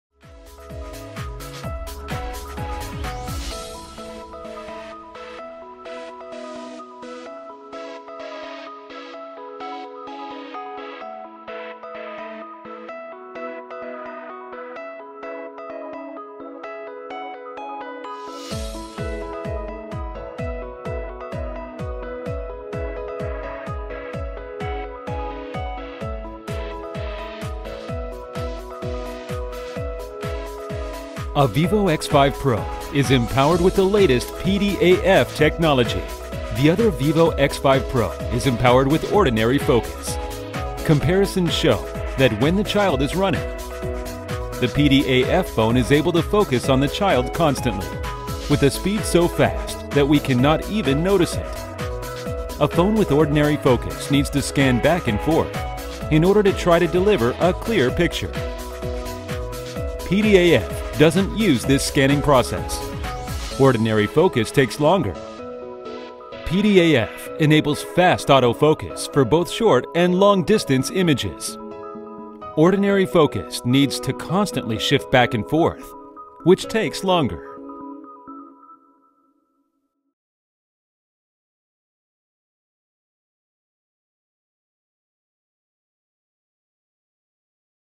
vivo X5Pro | Camera PDAF Test
This video will introduce how PDAF technology works on the vivo X5Pro. And it will also contrast the X5Pro in PDAF mode and Ordinary focus mode.